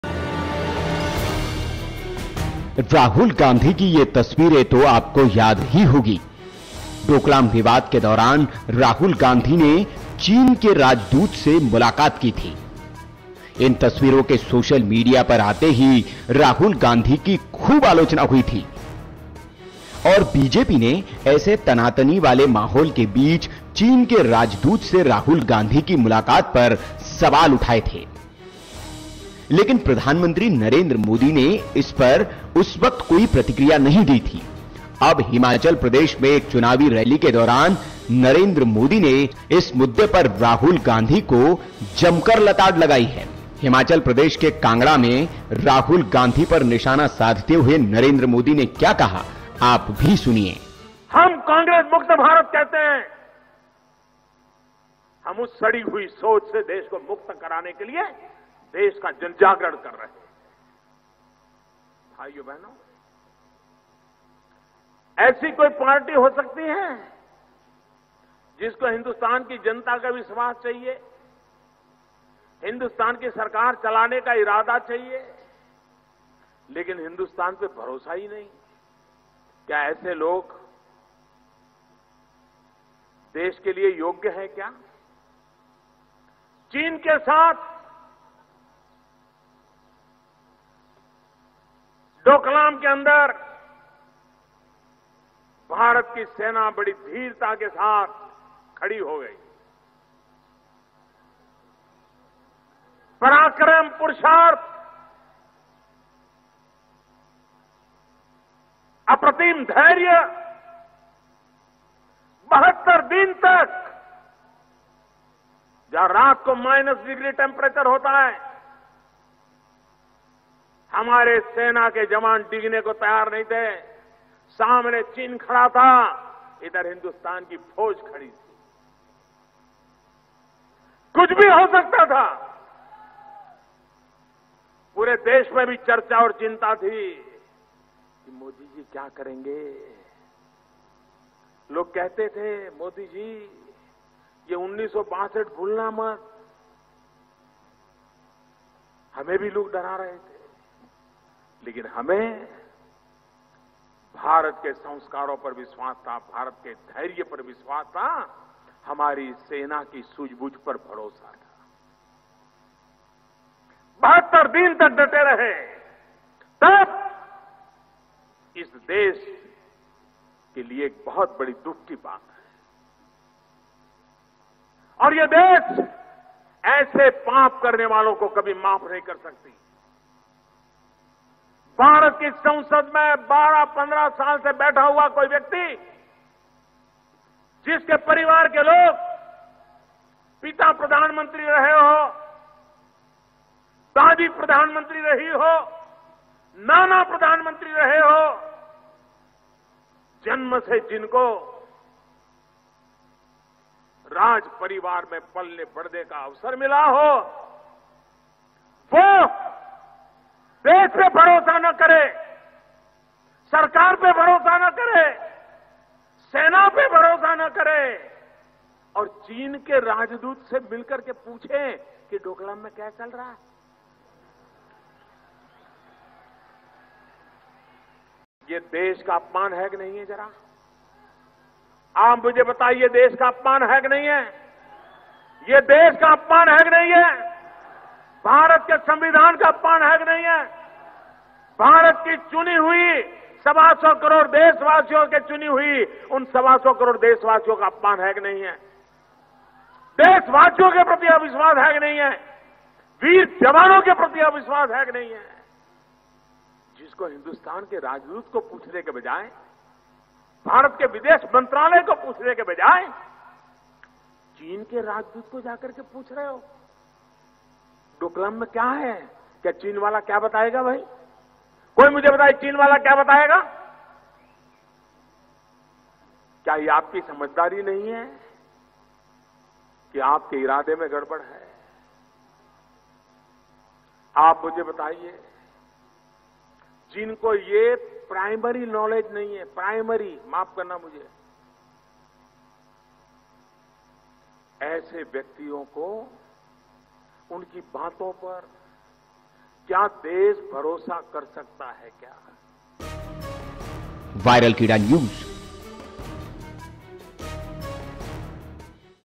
राहुल की नियत पर मोदी को शक, पूछा- चीनी राजदूत से क्यों मिले|PM Narendra Modi attacks Rahul Gandhi
Hitting out at Rahul Gandhi for meeting the Chinese Ambassador during the recent border standoff at Doklam, Prime Minister Narendra Modi said on Thursday in Kangra Himachal Pradesh public rally that it was a matter of “great sadness” that the Congress Vice-President did not “trust the country or its government” during that period.
Addressing an election rally in poll-bound Himachal Pradesh at Rehan in Kangra district, home to a sizeable number of current and former members of the armed forces, Modi praised the courage and “sensible approach” of the Indian Army under difficult circumstances during the border face-off with China.

tweeter-
Google+-